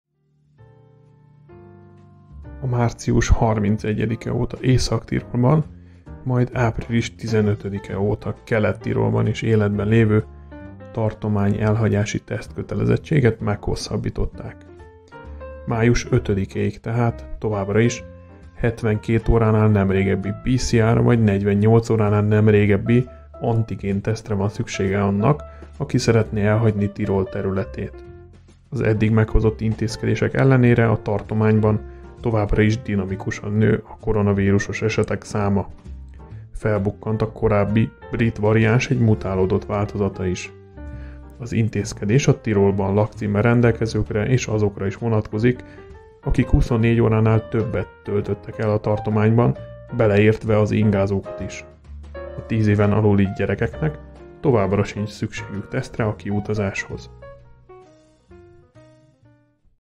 Meghosszabbították a kiutazási tesztkötelezettséget Tirolban.
Kövesd az AusztriaInfo-t podcast formátumban is Spotifyon vagy Google Podcastsen.

2021 április 25.